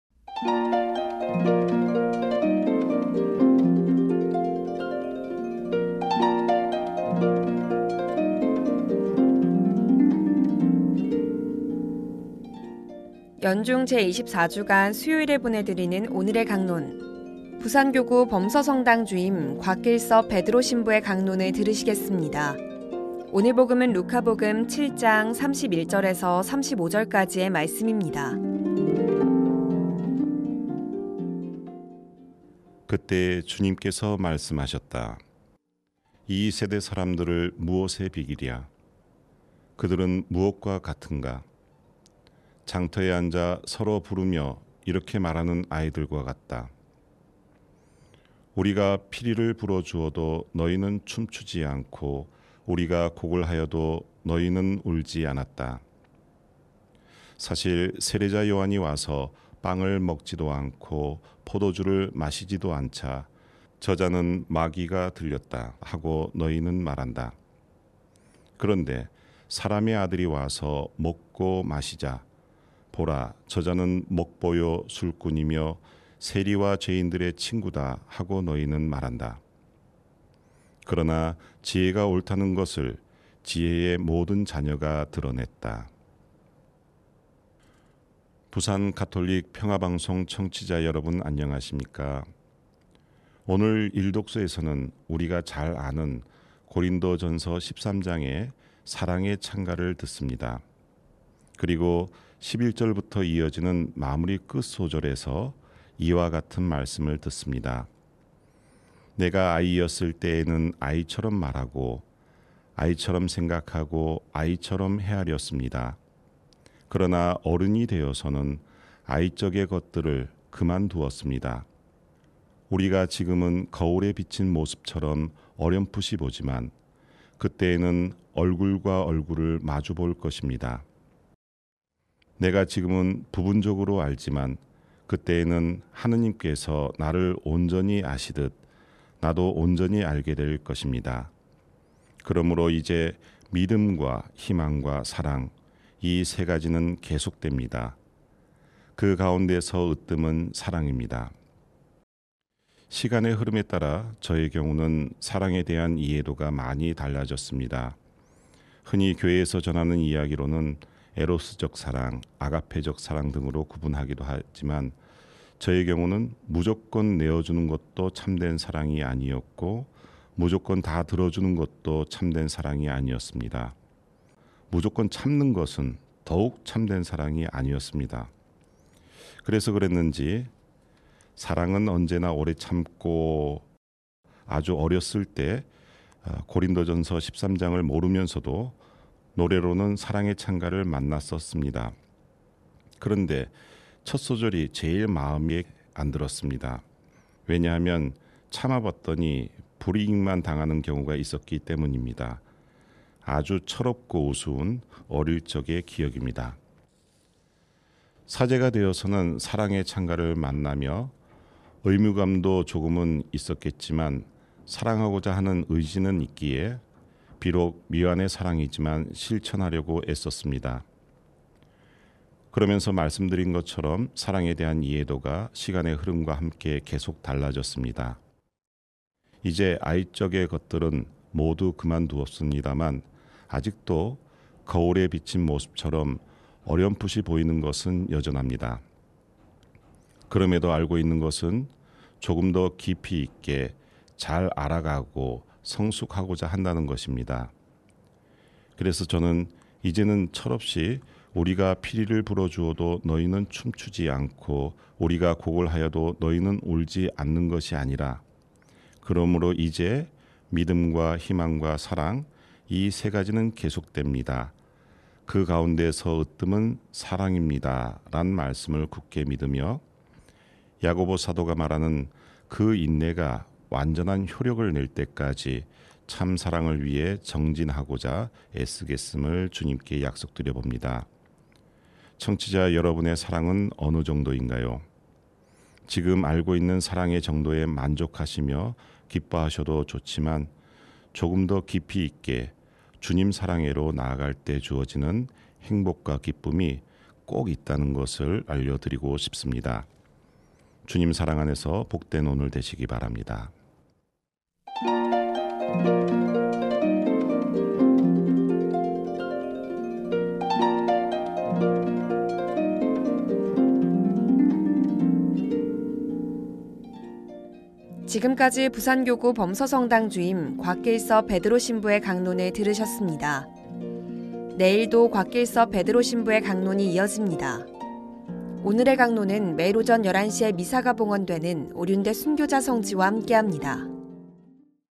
오늘의 강론(2024.09.18) - 부산교구 범서성당 주임 곽길섭 베드로 신부
✠ 루카가 전한 거룩한 복음                                                                               7,31-35
그때에 주님께서 말씀하셨다.
“이 세대 사람들을 무엇에 비기랴? 그들은 무엇과 같은가?
장터에 앉아 서로 부르며 이렇게 말하는 아이들과 같다.
‘우리가 피리를 불어 주어도 너희는 춤추지 않고
우리가 곡을 하여도 너희는 울지 않았다.’
사실 세례자 요한이 와서 빵을 먹지도 않고 포도주를 마시지도 않자,
‘저자는 마귀가 들렸다.’ 하고 너희는 말한다.
그런데 사람의 아들이 와서 먹고 마시자,
‘보라, 저자는 먹보요 술꾼이며 세리와 죄인들의 친구다.’ 하고 너희는 말한다.
그러나 지혜가 옳다는 것을 지혜의 모든 자녀가 드러냈다.”